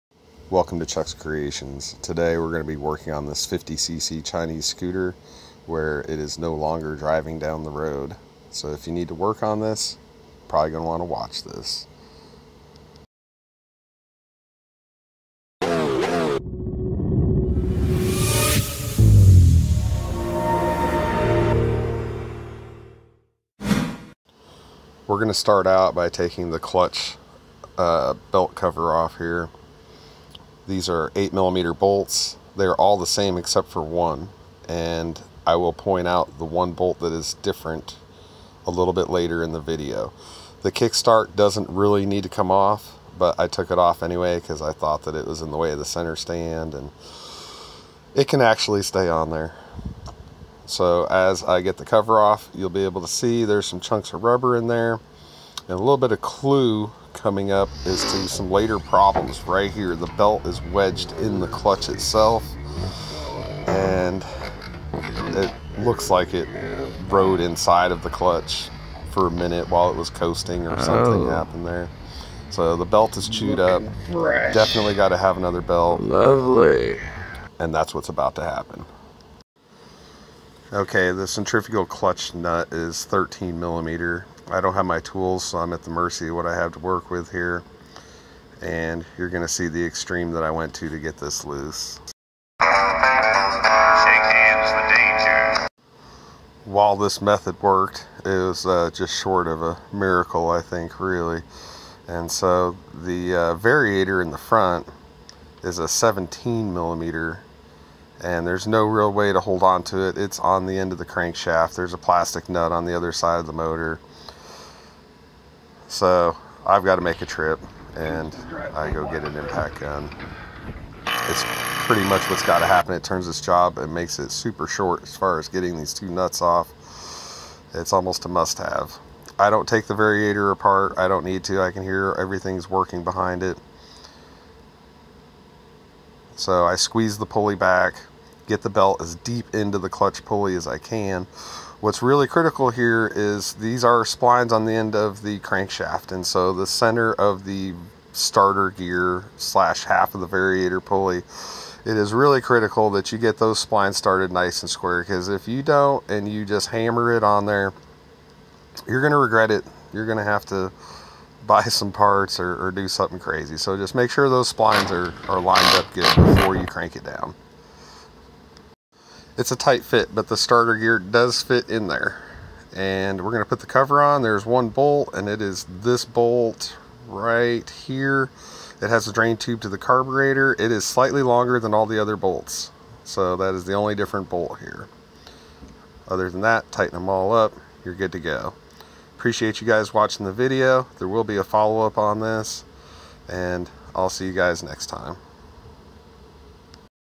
49cc Scooter Belt Replacement
How to change the drive belt on a 49/50cc Chinese scooter